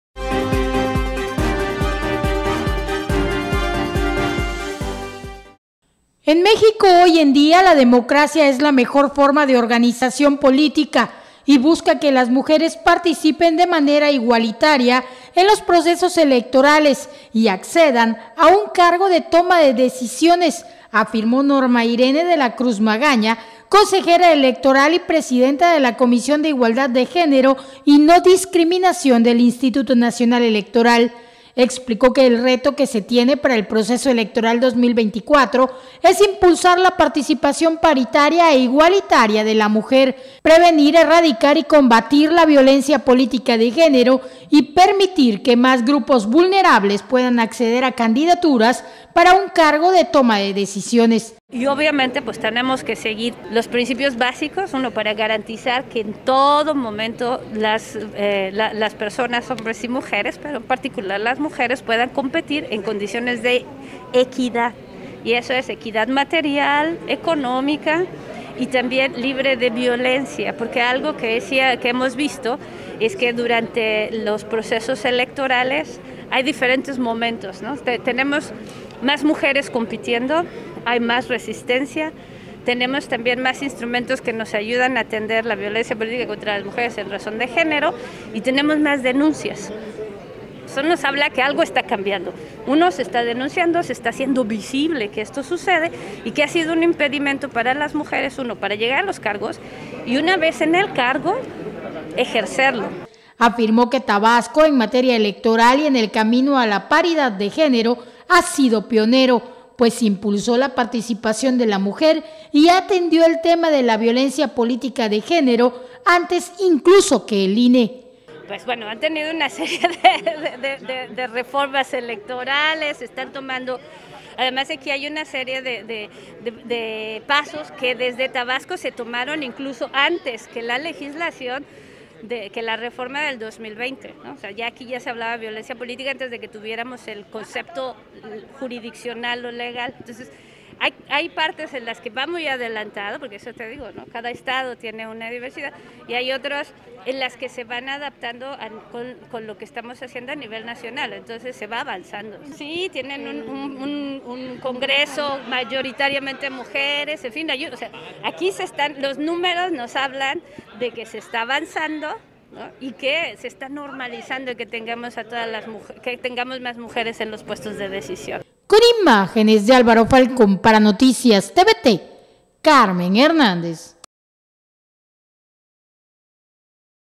Tabasco ha sido pionero a nivel nacional en el impulso de la paridad de género
NoticiasTVT🟣 Con sus reformas electorales, Tabasco ha sido pionero a nivel nacional en el impulso de la paridad de género, incluso antes que el Instituto Nacional Electoral, destaca la consejera del INE, Norma Irene de la Cruz.